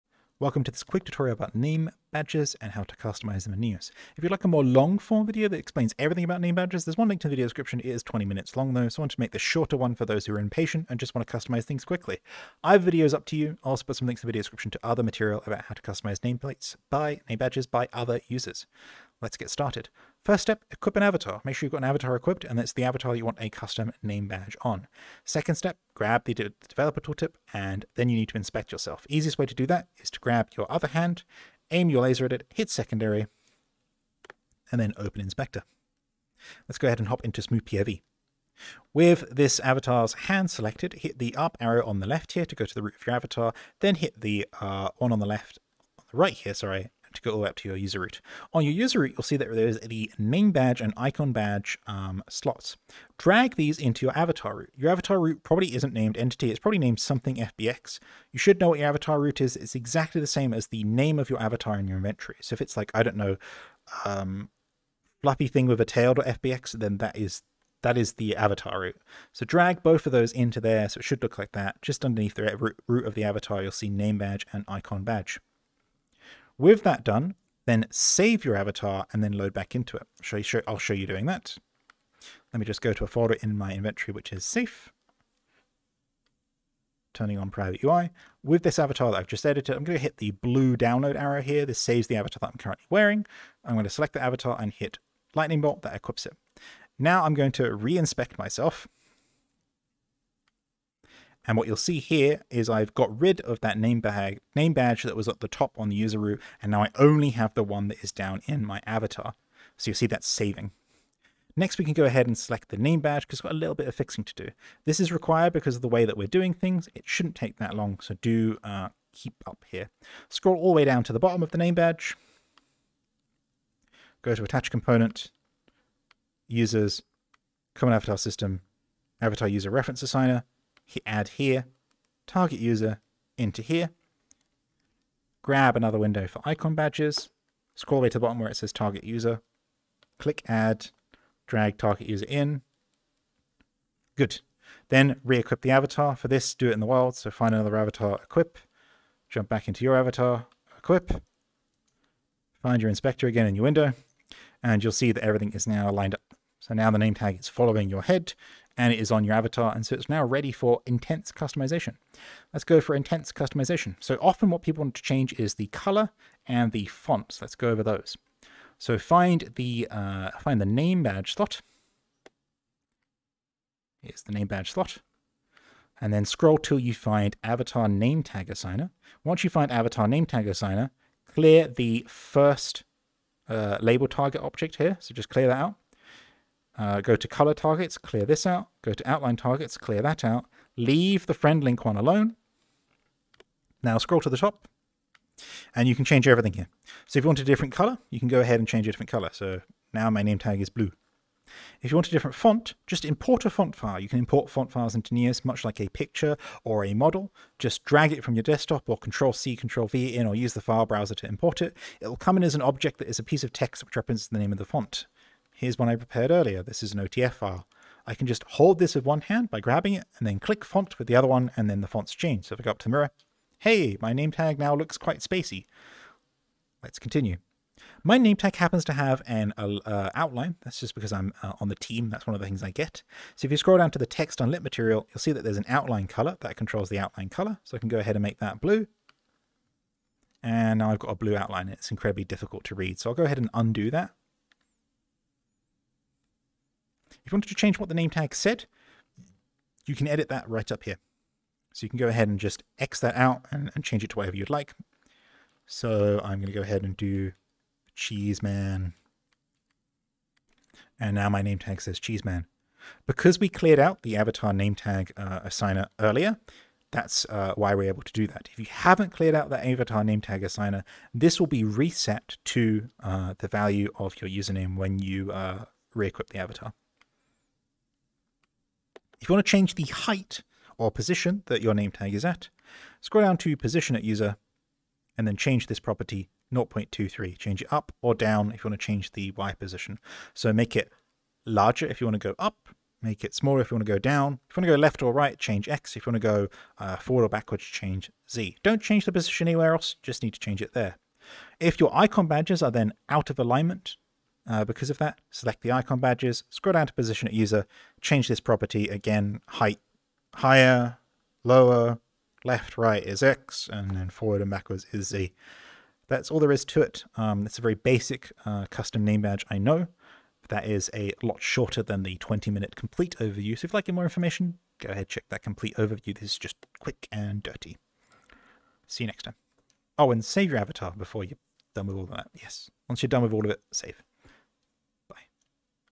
OLD: Neos Quick Tutorial: Name Badges
After recording the previous video which provides an in depth overview of name badges, I noticed it was 20 minutes long. This is a re-recording of it, cutting any extraneous detail.

If you don't understand something you see, you might need the previous video. Otherwise here you go :D

00:00 Introduction
00:20 Stealing an existing Name Badge
01:50 Fixing the stolen Name Badge
02:42 Unlocking the Colors, Text etc
03:15 Changing Colors
03:22 Changing Fonts
03:49 Changing Outlines
04:08 Changing the Text
04:39 Changing the Height of the Name Badge
05:00 Changing the Icon Badges Height
05:15 Conclusion

Yay, these thingies(name badges) have a lot of names.

Name Tags, NameTag,namebadge, namebadges, name, badge

Grab my Public Folder by copying and pasting this string into your Neos Window: neosrec:///U-ProbablePrime/R-0776eec6-e4d1-46b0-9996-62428b1310b0